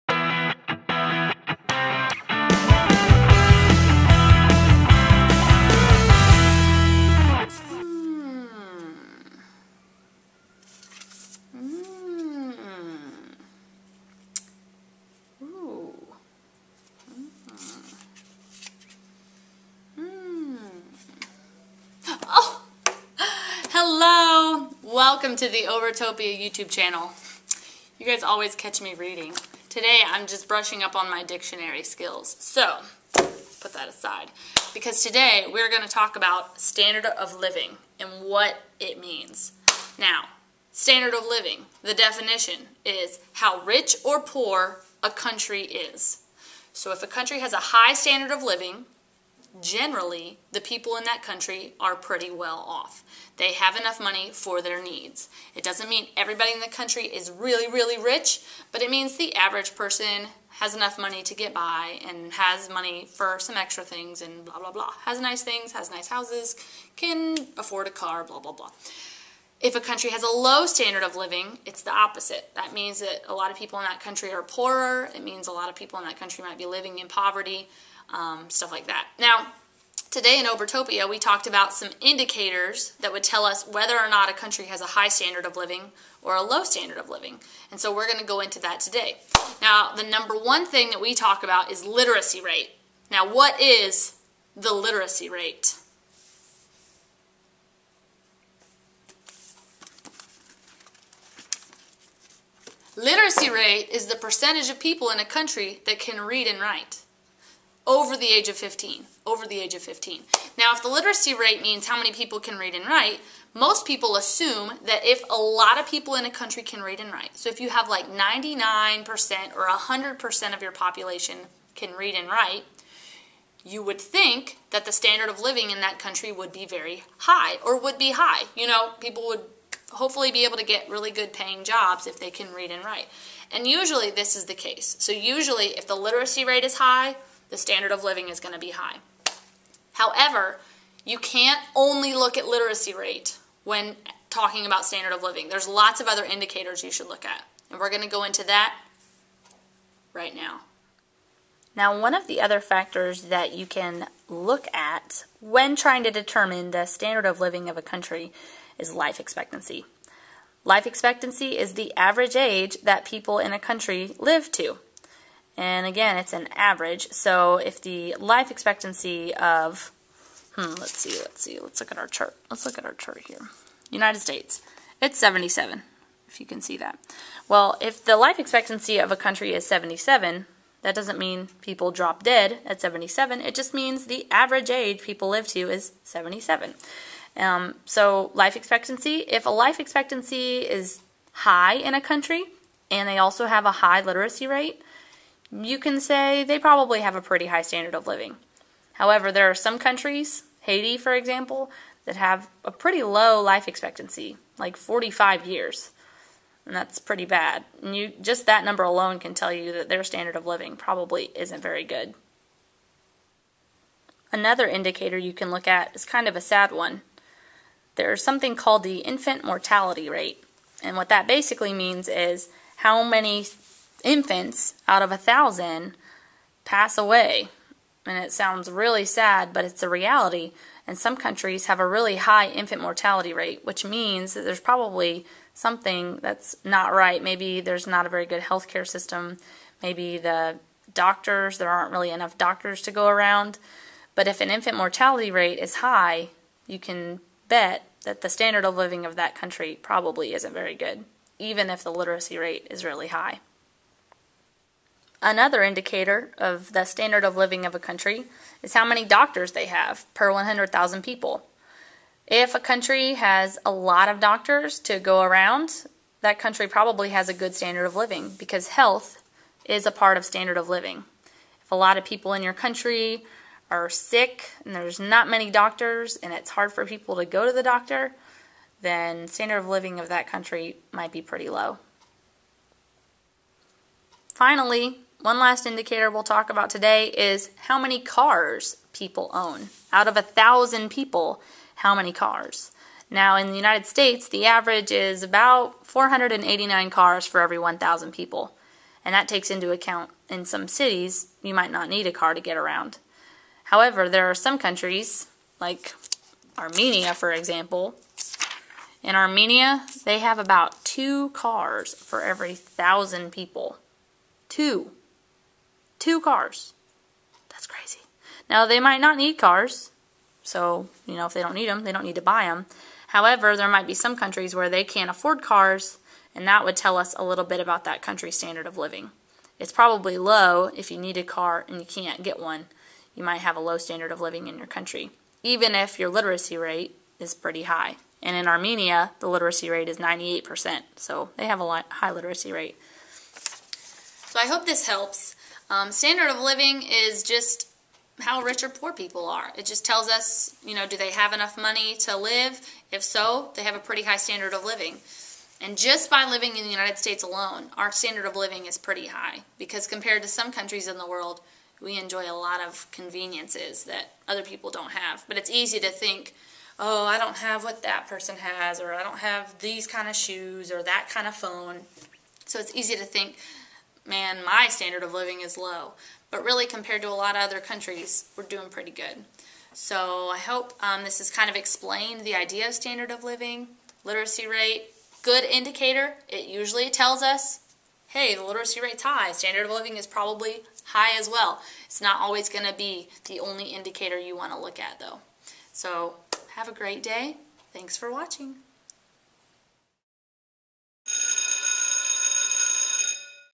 Standard of Living
The literacy rate is usually a good indicator of a country's standard of living.  However, it shouldn't be the only factor we look at when determining how rich or poor a country might be.  Watch this video for some more standard of living indicators!